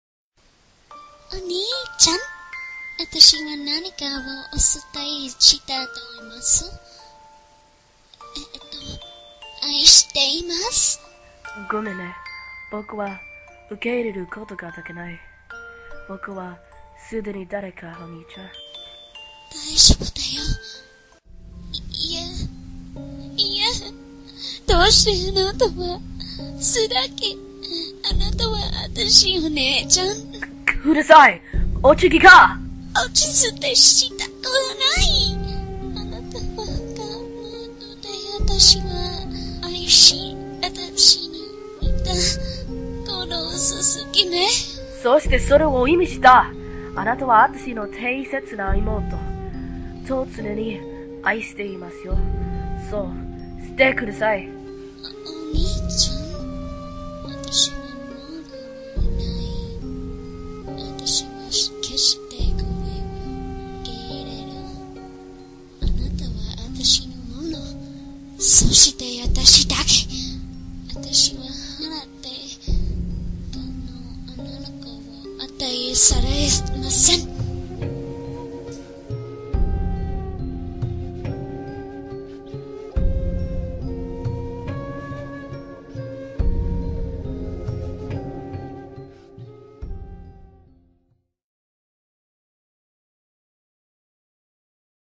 "Yandere Imouto" 4 (Cake-ChanxMaiya-Chan)
So the series continues for people who hadn't quit at the first...

This time, dear ol' Onii-Chan (Maiya-Chan) has found someone and poor imouto (Cake-Chan- me) is stuck as a forever alone yandere bi-...Beach.

So... Welcome back Cake as rising from dead.

Script written by yours truly. Music is mainly from Seventh Expansion and I claim no ownership.